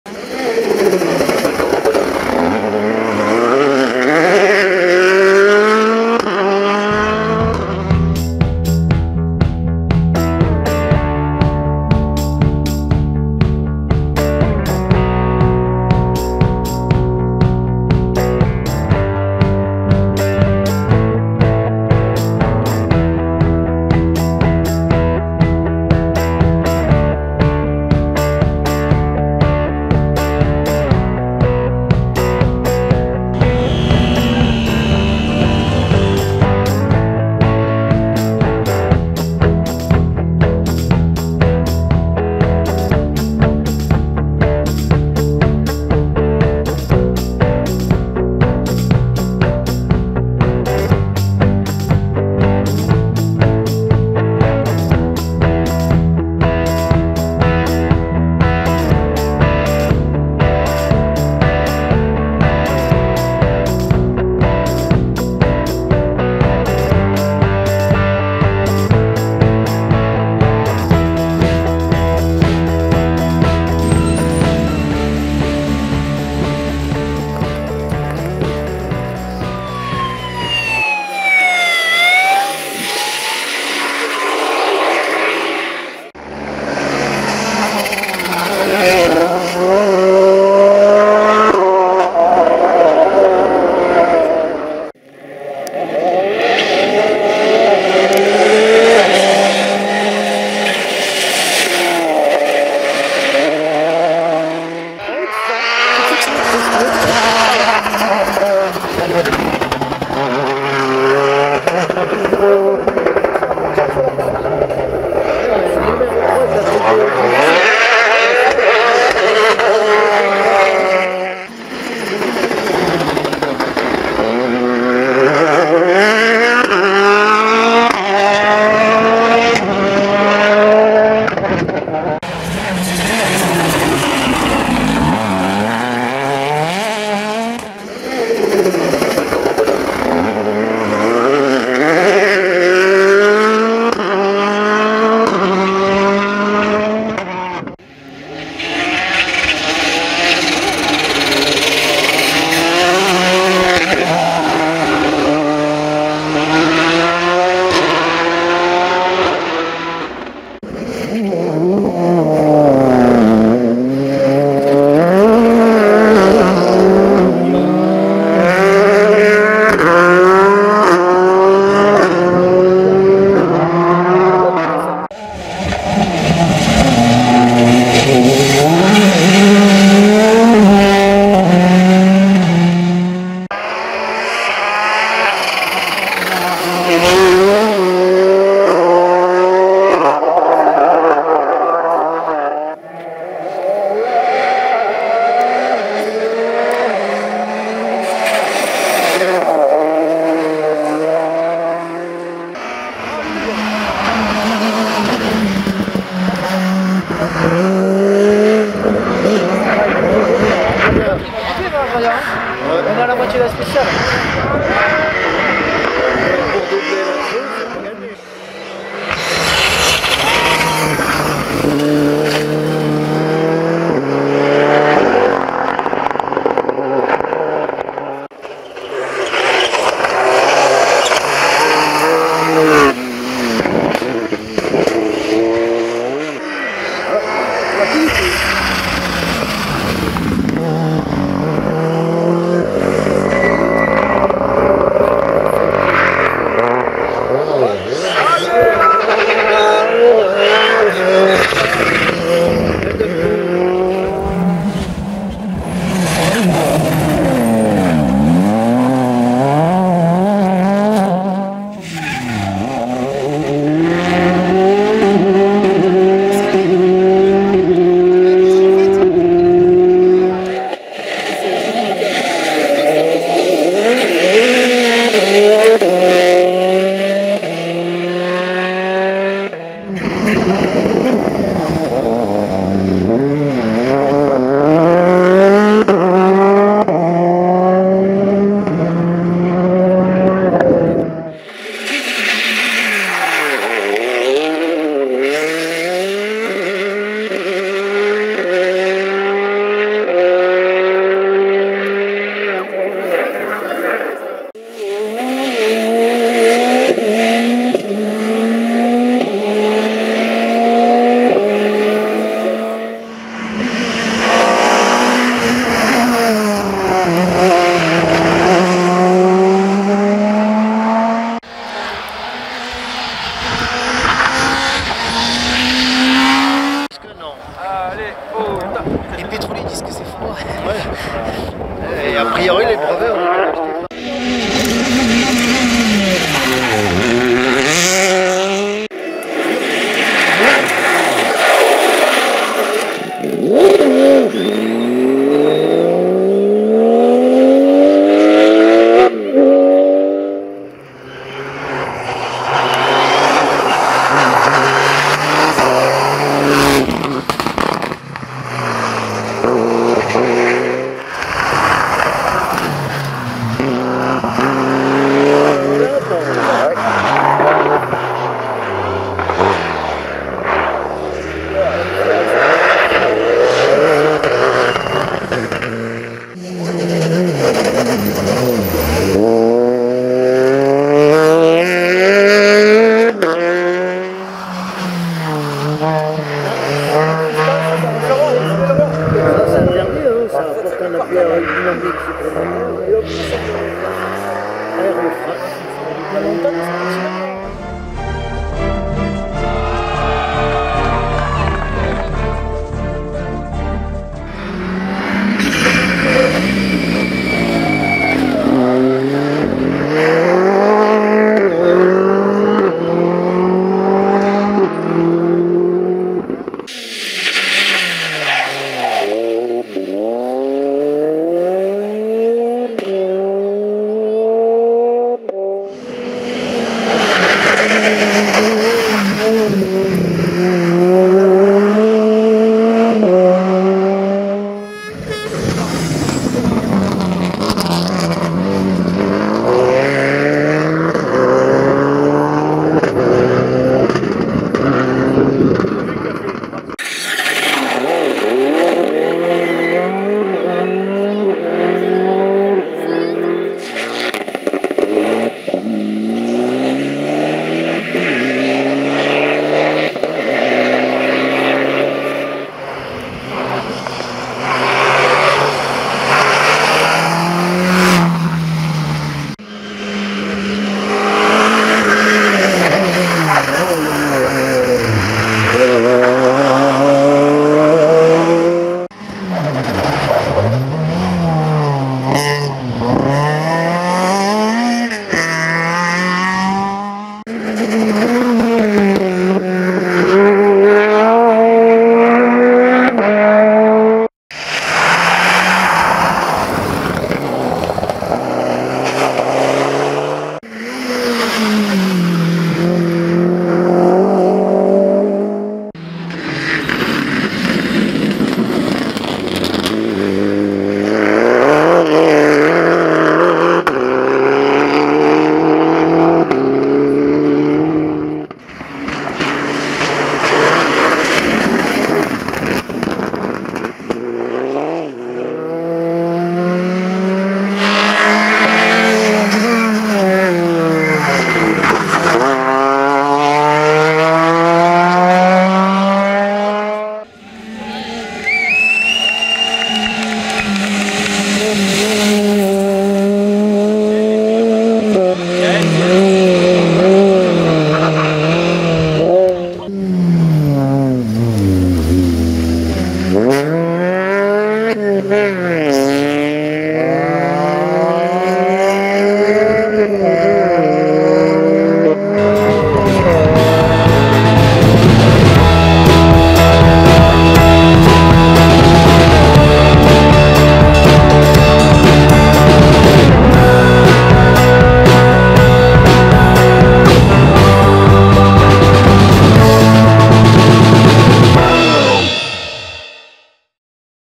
Rallye Monte Carlo 2014 Best of N°2
Les meilleurs passages filmés par Team3V01 du premier au dernier et ceux des malchanceux. Victoire de Sébastien Ogier et Julien Ingrassia su VW Polo WRC.